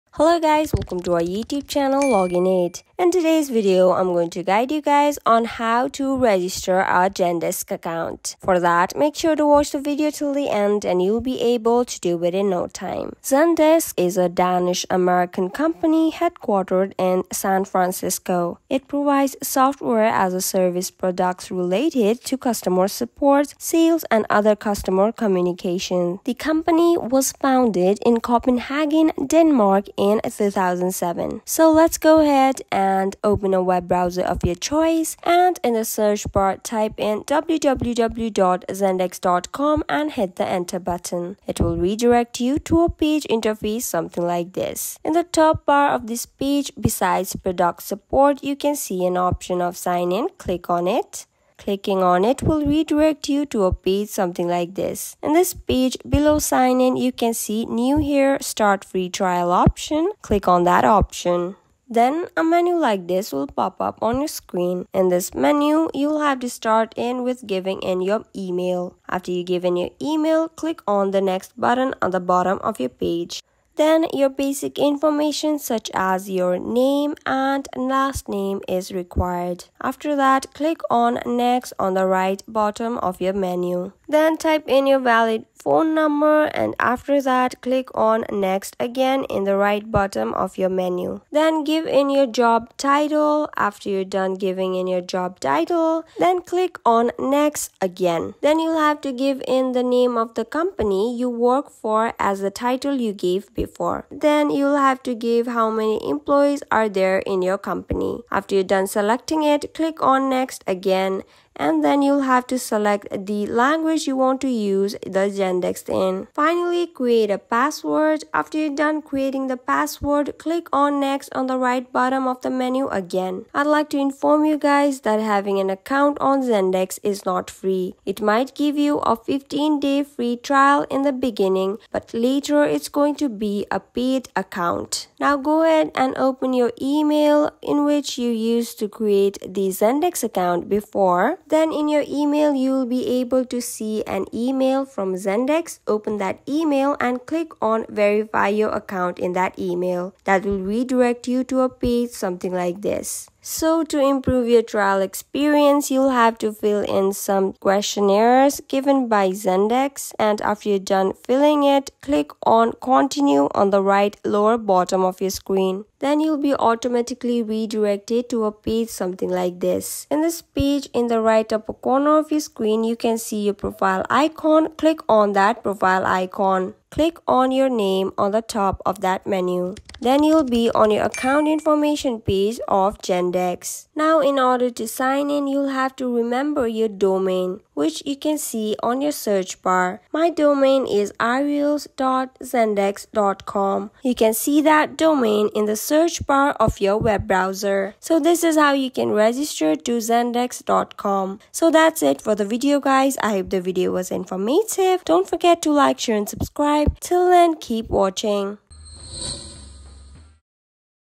Zendesk Account Registration, Sign Up Guide 2023 | Create Zendesk Account
A video guide for the process to create Zendesk account.